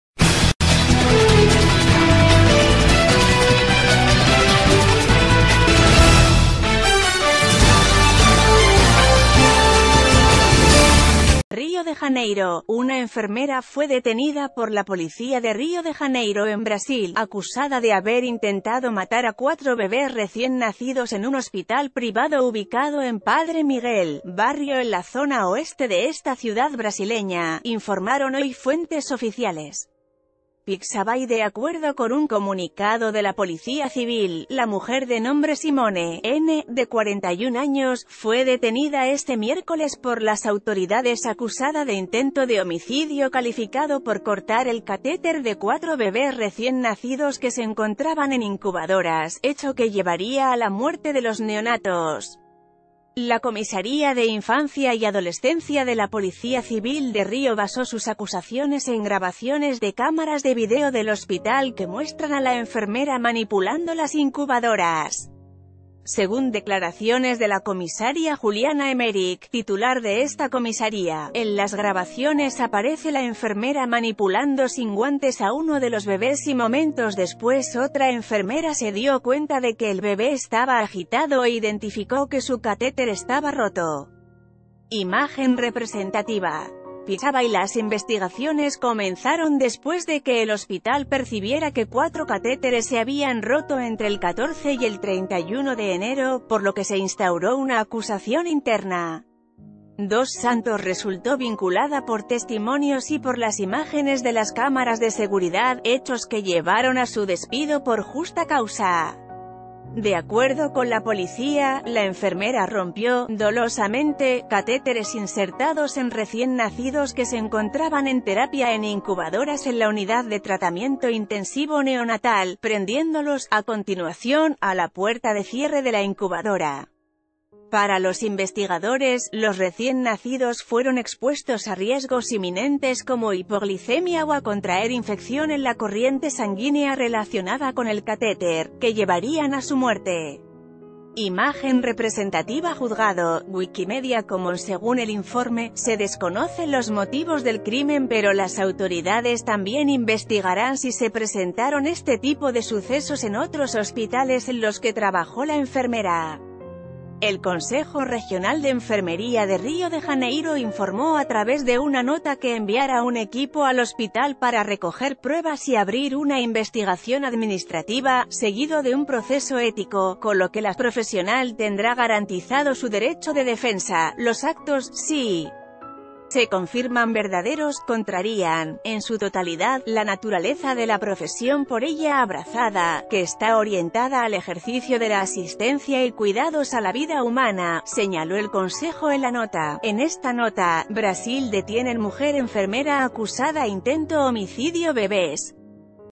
Detiene a enfermera acusada de intentar matar a cuatro bebés
Río de Janeiro.- Una enfermera fue detenida por la Policía de Río de Janeiro en Brasil,  acusada de haber intentado matar a cuatro bebés recién nacidos en un hospital privado ubicado en Padre Miguel, barrio en la zona oeste de esta ciudad brasileña, informaron hoy fuentes oficiales. Foto. PixabayDe ...